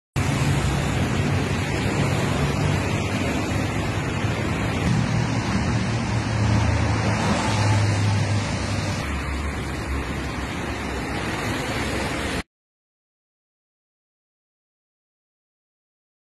Colocan refuerzo de acero en viaducto Juárez Serdán por riesgo de colapso
Hace dos semanas se reportó que una columna de concreto del viaducto Juárez- Serdán, presentaba desprendimiento y grietas por el peso de la ciclovía elevada.